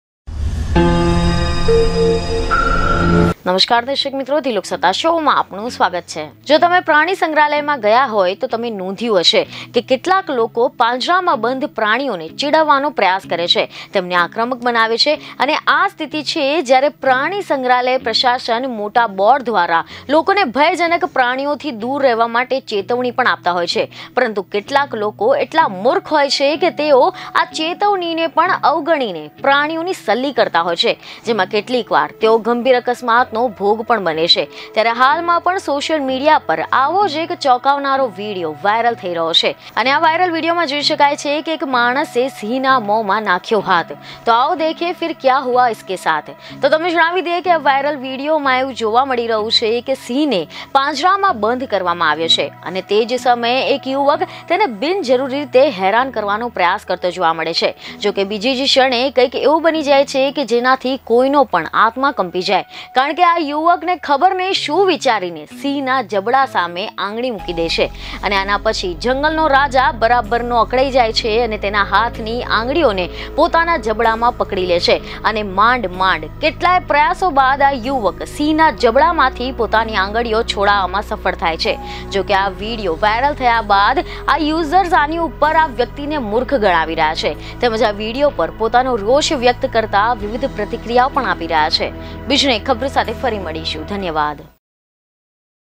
સિંહના મોઢામાં આંગળી નાખી દીધી! લોકો વીડિયો બનાવતાં રહ્યાં! | Viral Video
આ યુવક ખબર નહીં શું વિચારીને સિંહના જડબા સામે આંગળી મૂકી દે છે. આ પછી, જંગલનો રાજા બરાબરનો અકળાય છે અને તેના હાથની આંગળીઓને પોતાના જબડામાં પકડી લે છે. અને માંડ માંડ કેટલાય પ્રયાસો બાદ આ યુવક સિંહના જબડા માંથી પોતાની આંગળીઓ છોડાવવામાં સફળ થાય છે.